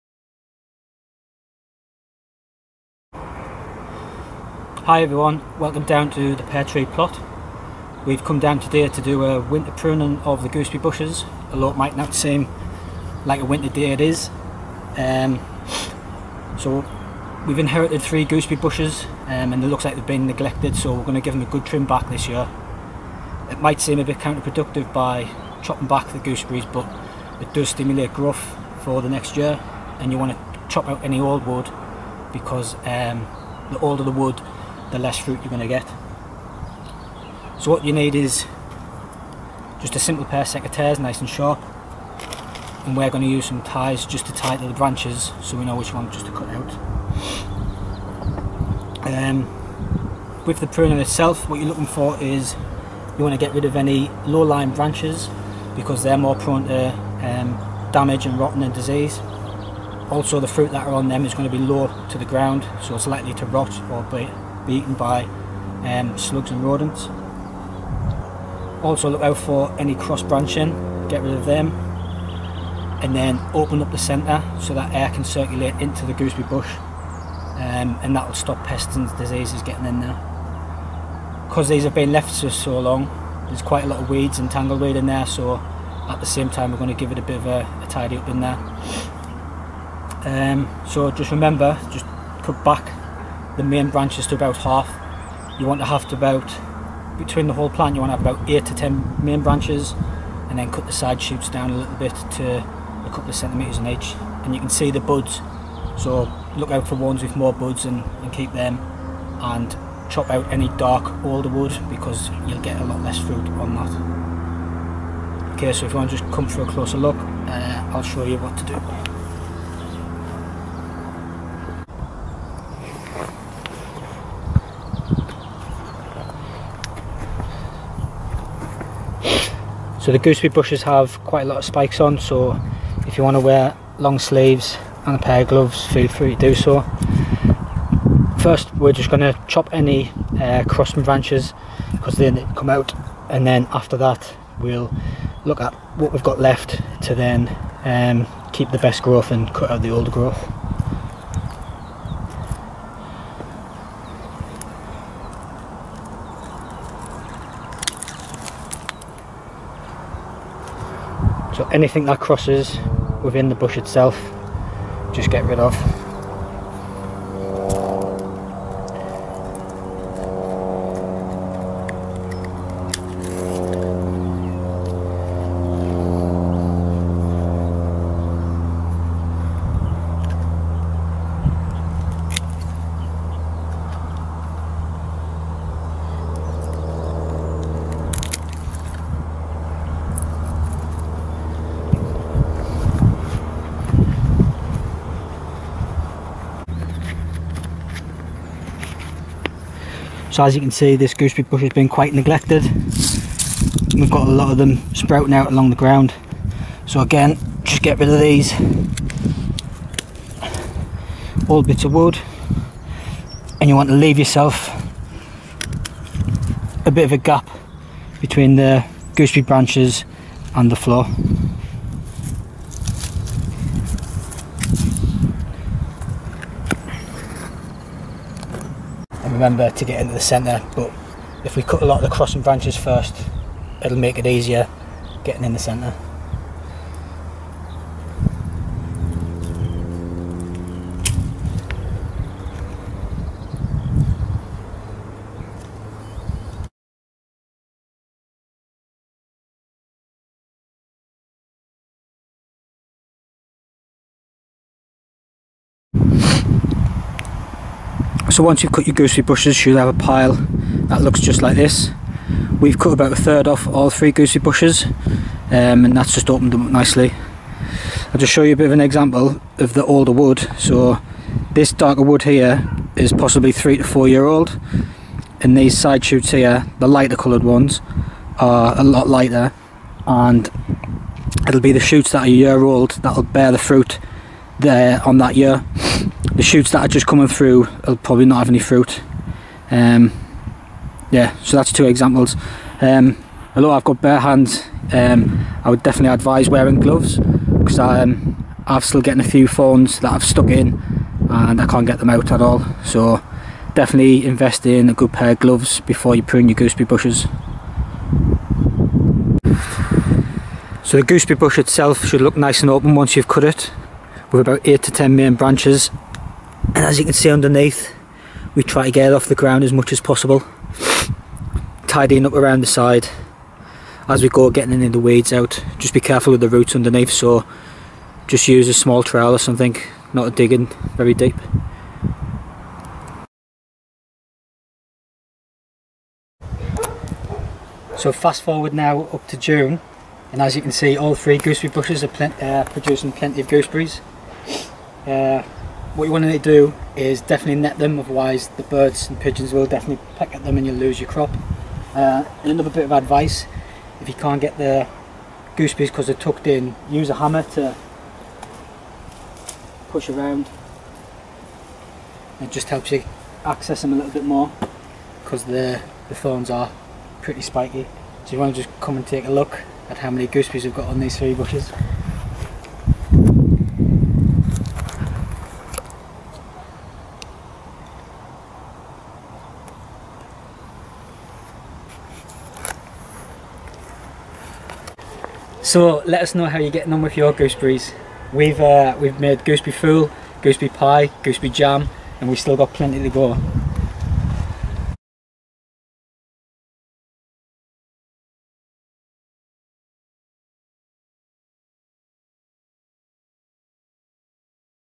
How to prune your Gooseberry bush
We have uploaded this short gooseberry pruning video for you to use to keep your bushes healthy and producing as much fruit as possible, showing you important and easy steps to take.

We pruned ours in February as a winter prune when the bush is dormant, they were in desperate need of it and had been left several years. It was our first time pruning and we were apprehensive at first chopping it back, but we have included some footage at the end showing you some of the results.